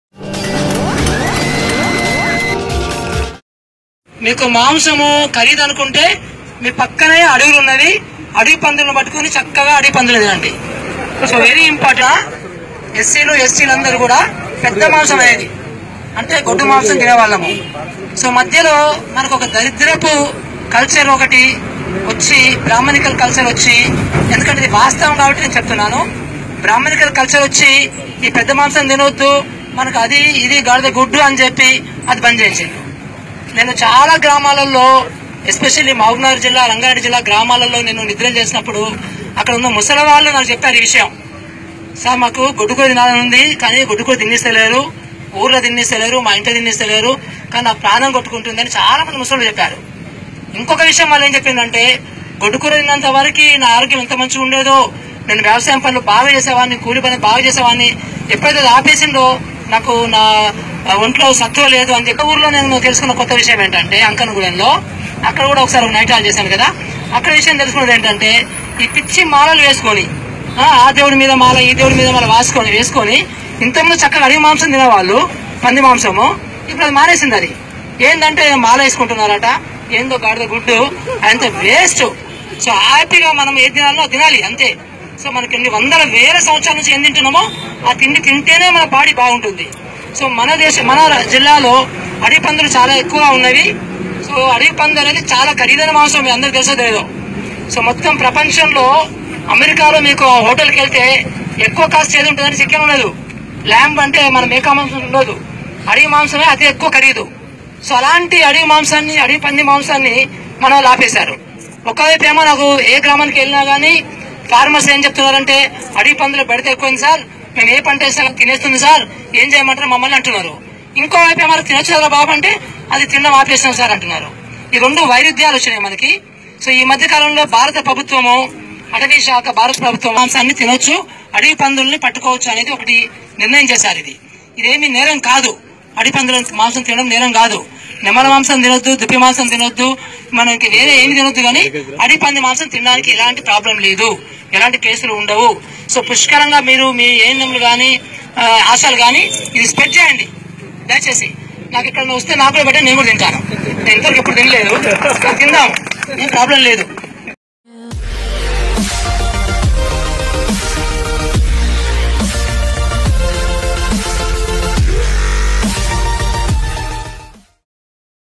Telangana collector blames it on "dirty Brahminical culture" || News Cabin
The video clipping shows A Murali, the collector of Jayashankar Bhupalpalli district, making the sensational comment at a rally at the tribal village of Eturu Nagaram on the occasion of World Tuberculosis Day on Friday.
                      The gatherer was alluding to the prohibition on utilization of meat of wild pigs and pork for the sake of religious beliefs. "I was told wild hogs have been obliterating crops in these tribal regions. In any case, I am letting you know, you can execute these pigs and pigs and joyfully eat their meat.